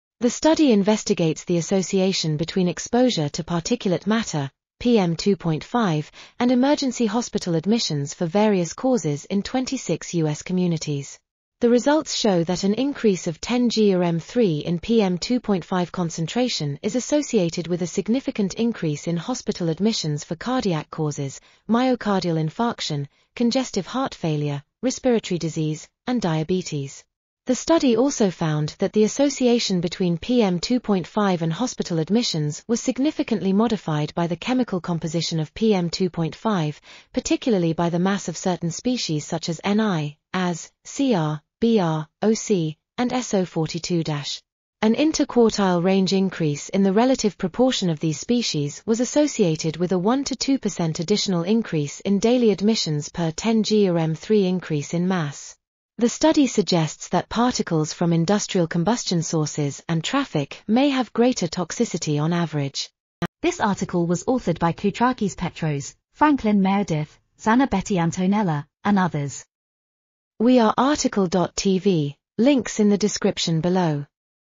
Fine particulate air pollution and its components in association with cause-specific ... | RTCL.TV
### Keywords ###

### Article Attribution ###
Title: Fine particulate air pollution and its components in association with cause-specific emergency admissions
Authors: Koutrakis Petros, Franklin Meredith, Zanobetti Antonella ,and Schwartz Joel
Publisher: BMC
DOI: 10.1186/1476-069X-8-58

### Video Timestamps ###
0:00:00 - Summary
0:01:03 - Title
0:01:10 - Tail
0:01:14 - End